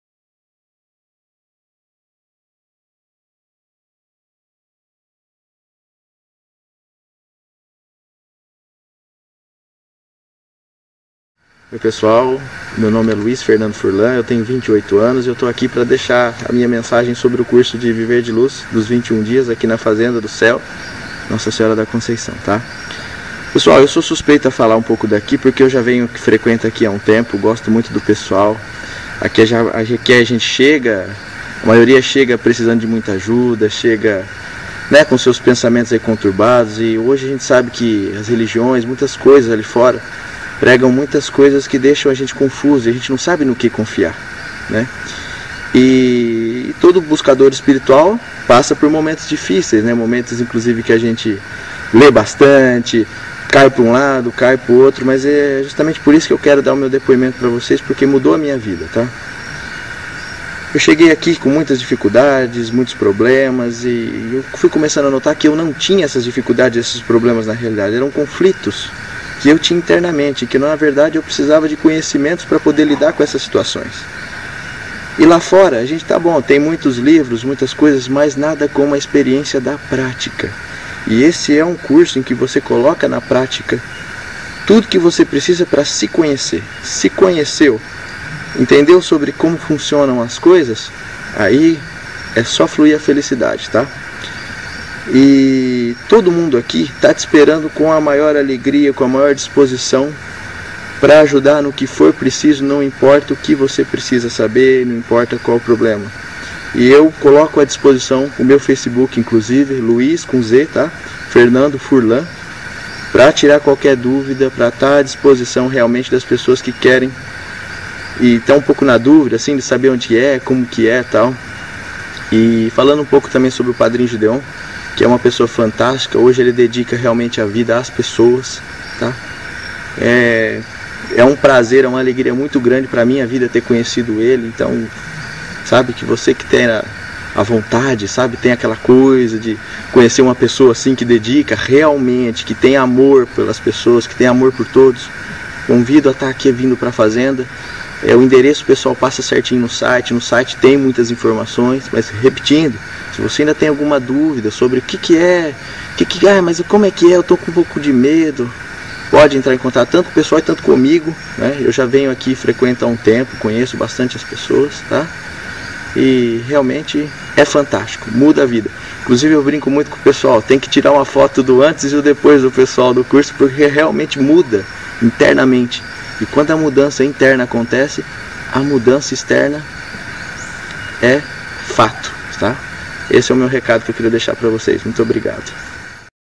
Depoimento Luiz Fernando - Viver de Luz Julho 2015
Depoimento de quem vivenciou o Curso Viver de Luz Consorciado ao Vegetarianismo Simbiótico, processo de 21 dias no Centro Espírita Ascensionado Céu Nossa Senhora da Conceição, durante o mês de Julho de 2015.